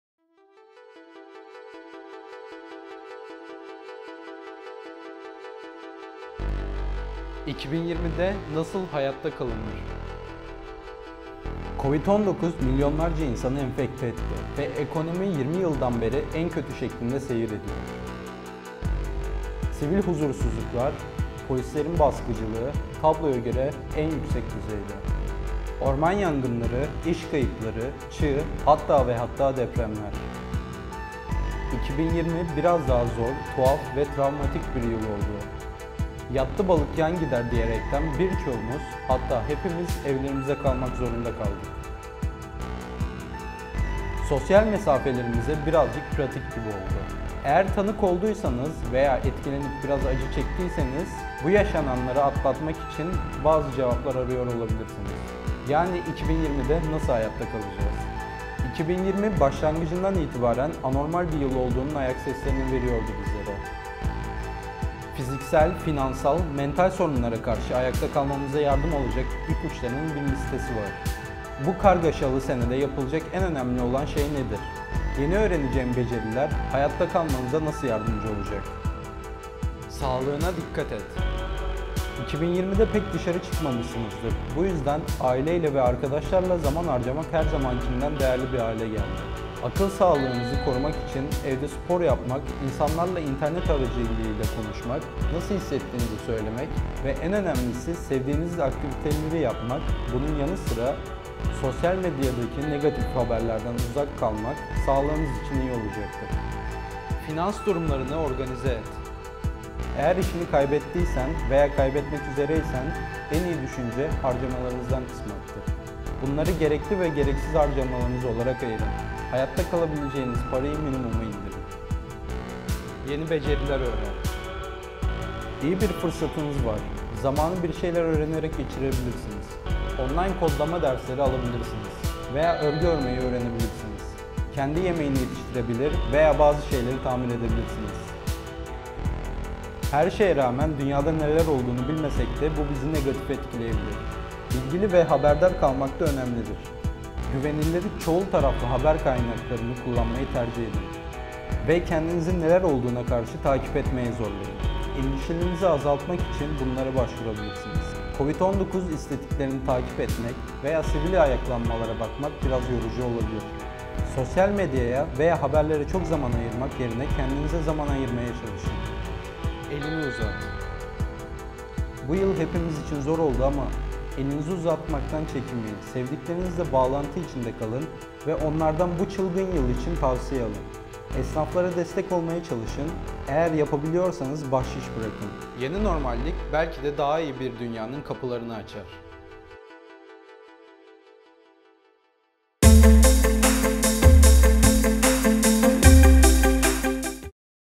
2020’de nasıl hayatta kalınır?
Kovid-19 milyonlarca insanı enfekte etti. Hepimiz bu süreç içerisinde evlerimizde kaldık. Dünyada ve ülkemizde birçok olumsuzluk yaşandı. Orman yangınları, iş kayıpları, çığ, depremler derken hepimiz için travmatik bir yıl oldu. Eğer bu deneyimleri yaşadıysanız ve olanlardan etkilendiyseniz, bu olayları atlatmak için bazı cevaplar arıyor olabilirsiniz. Fiziksel, finansal, mental sorunlara karşı ayakta kalmanıza yardımcı olacak ipuçlarının bir listesi var... Biz de SuperHaber ekibi olarak size ipuçlarını derledik…